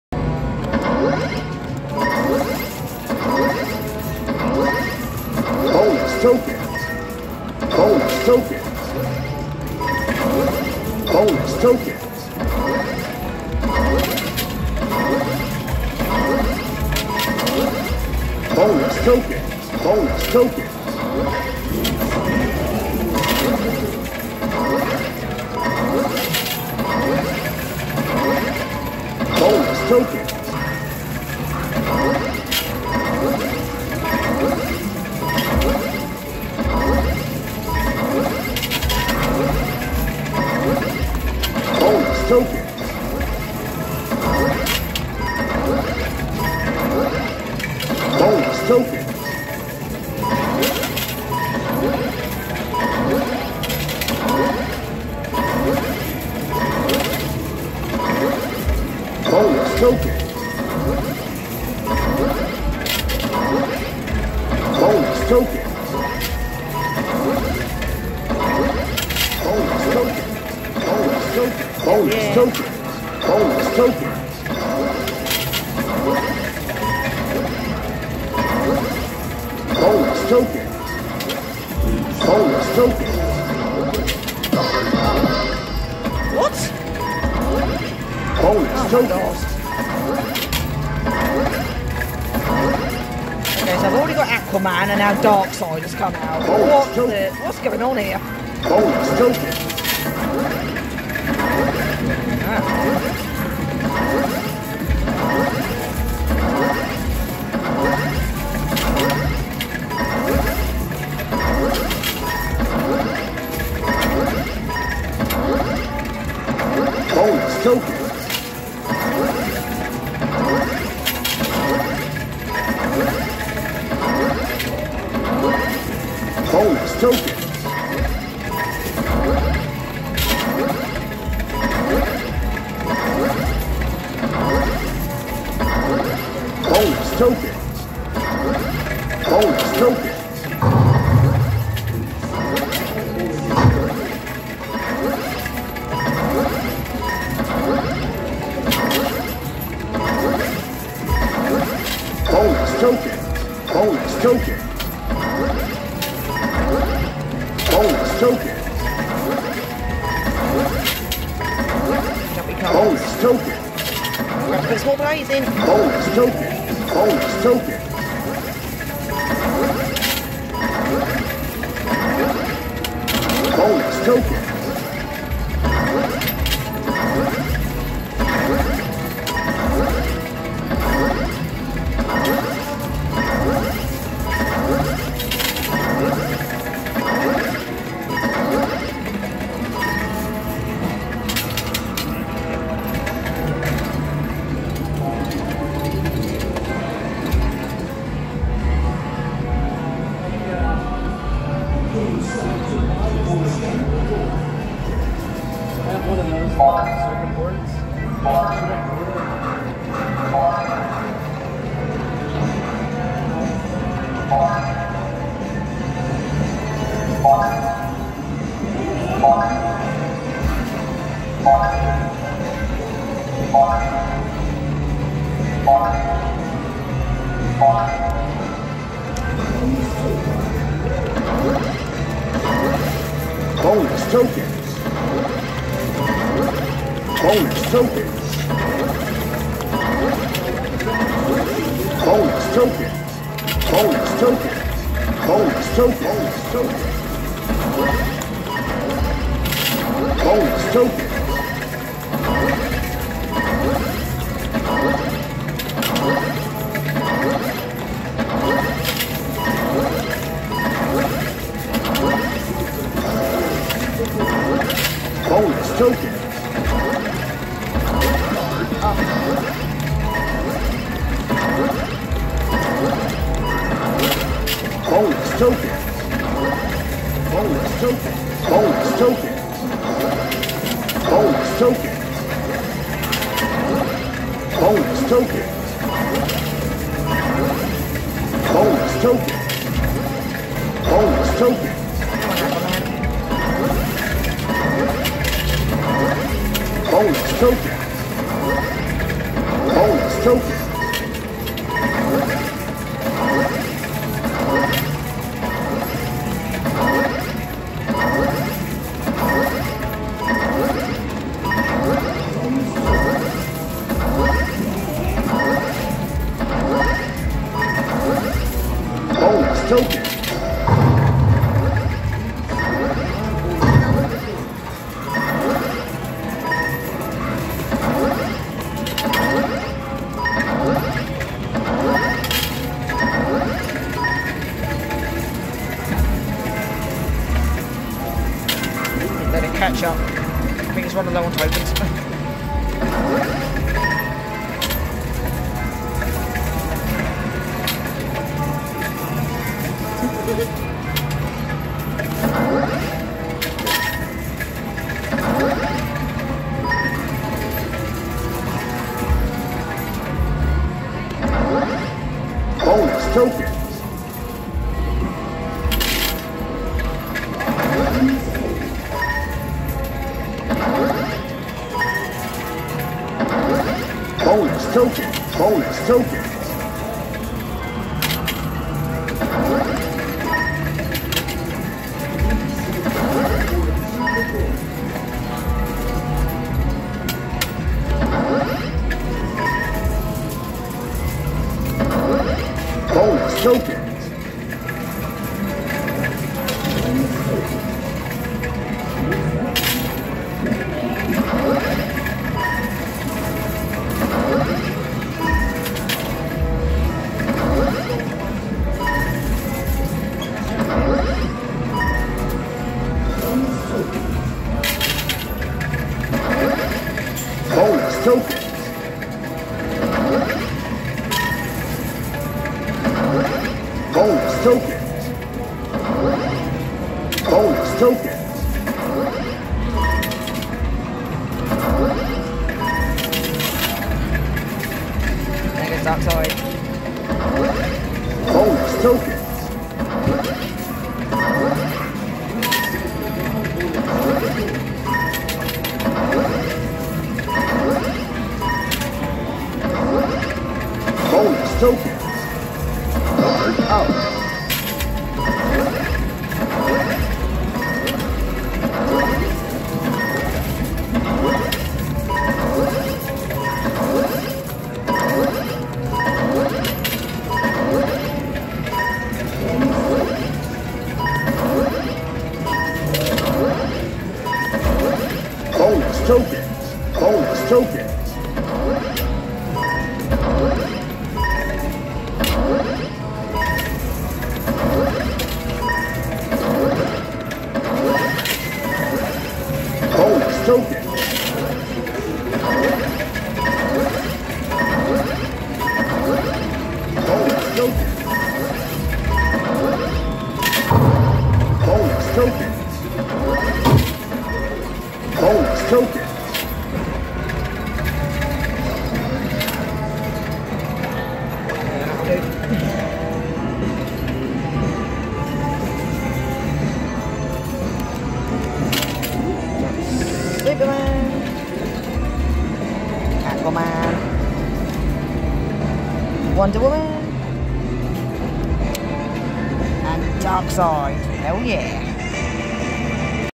What Is Happening?! | DC Comics Coin Pusher
Hey guys!
Decided to throw a few plays onto this DC Comics Coin Pusher,  as there was not one, not two but THREE Aquaman cards on the playing field! For those of you who aren't aware, Aquaman is the rare card for the heroes card set from this coin pusher :) After playing for a bit, something else amazing happened!

I played this machine at Neb's Funworld, in Oshawa, Ontario, Canada.